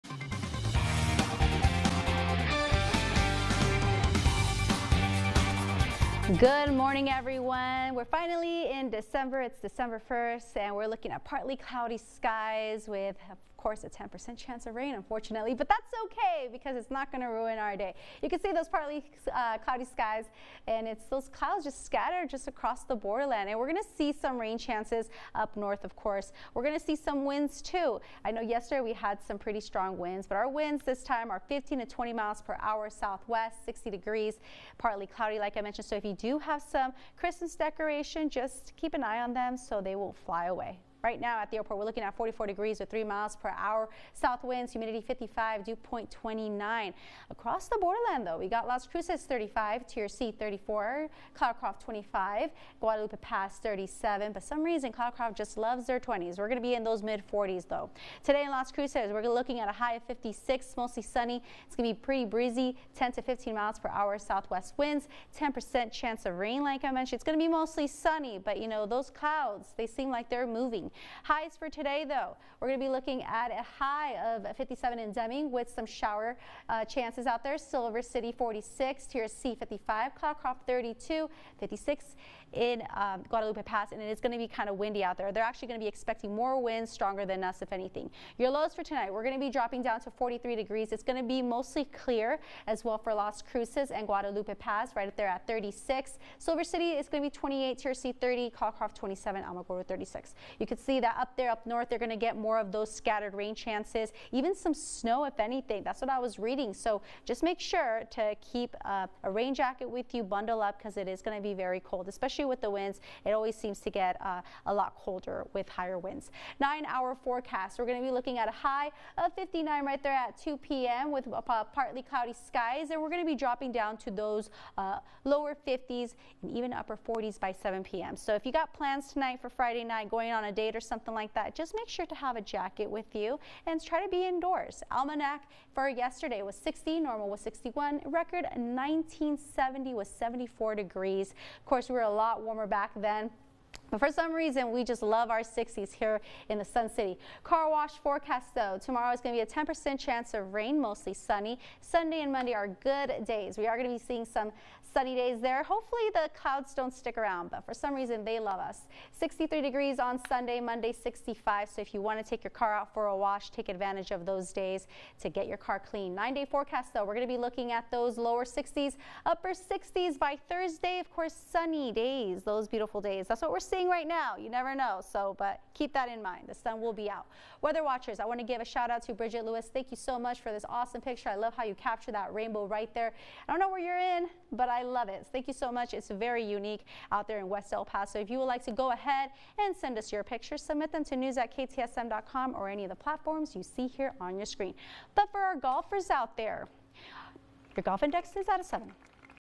Friday morning forecast with Jasmine Perry
Forecasting partly cloudy with rain chances today